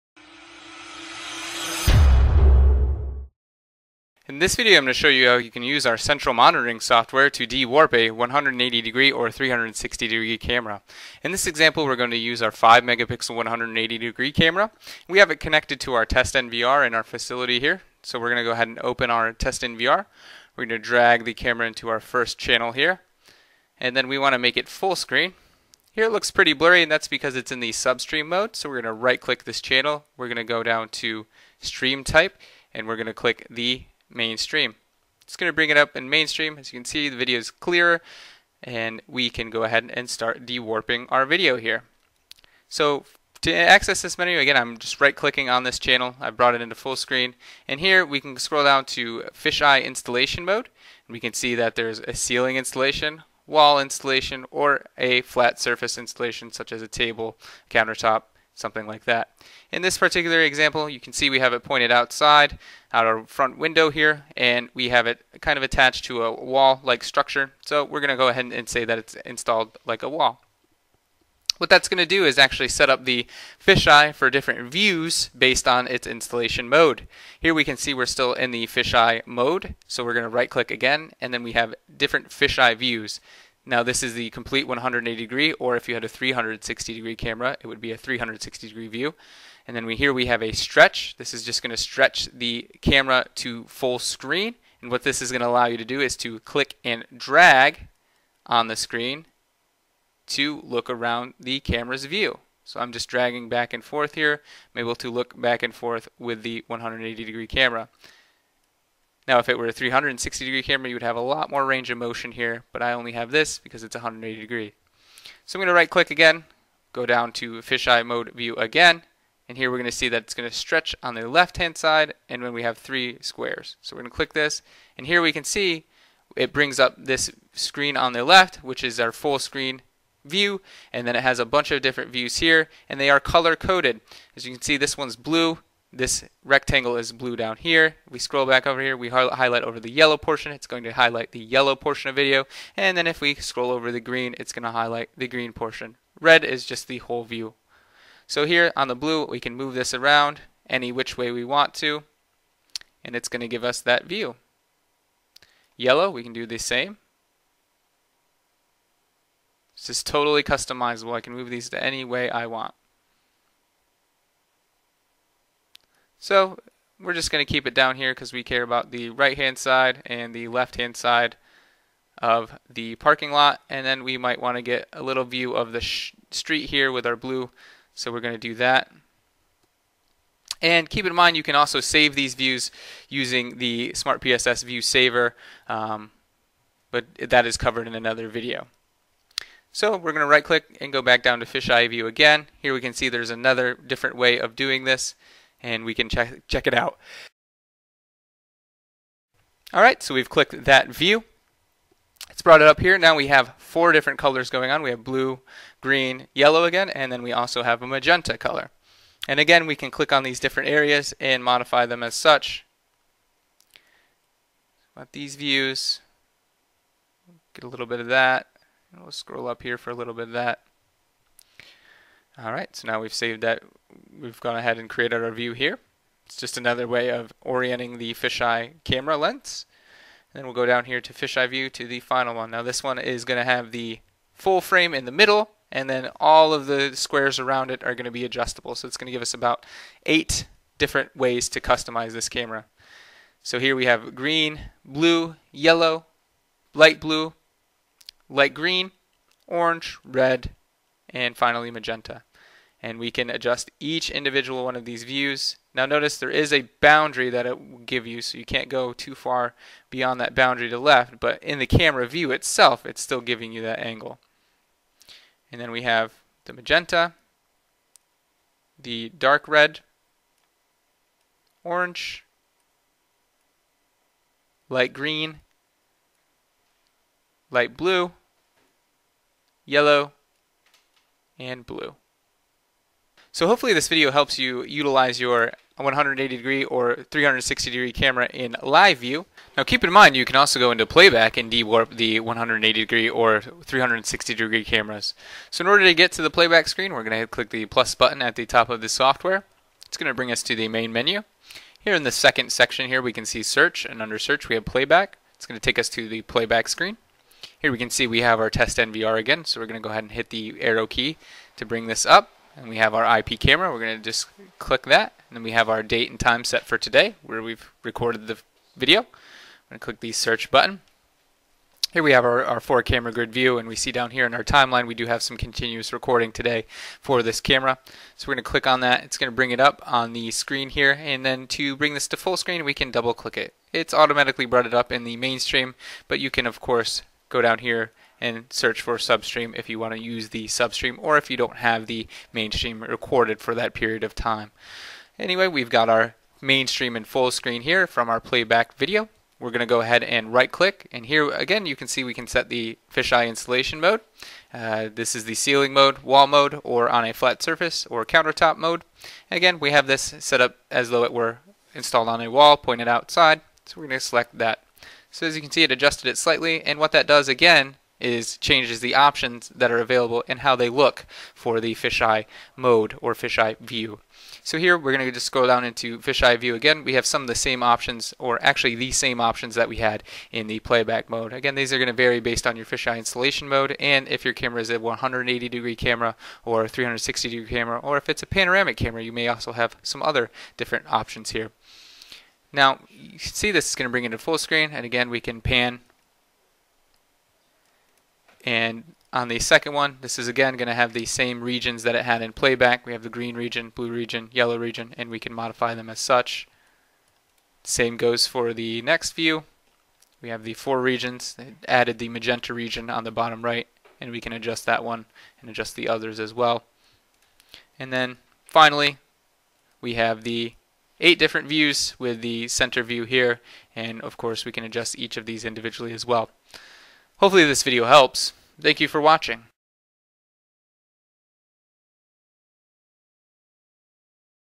How to View a Fisheye or Panoramic Camera using your Computer
In this video we show you how you can use our Central Monitoring Software on a PC computer to view and de-warp panoramic and fisheye camera.